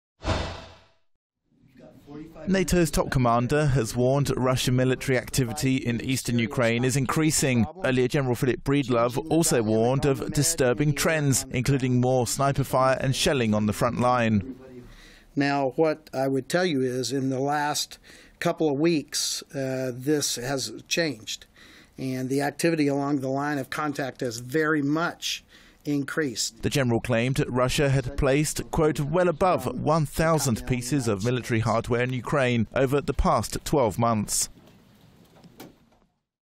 NATO Warning For Donbas: Russia is beefing up military activity in eastern Ukraine
The top NATO commander says more sniper fire and shelling has been reported in recent weeks

NATO's top commander has warned Russian military activity in eastern Ukraine is increasing. Earlier, General Philip Breedlove also warned of ‘disturbing trends' – including more sniper fire and shelling on the front line.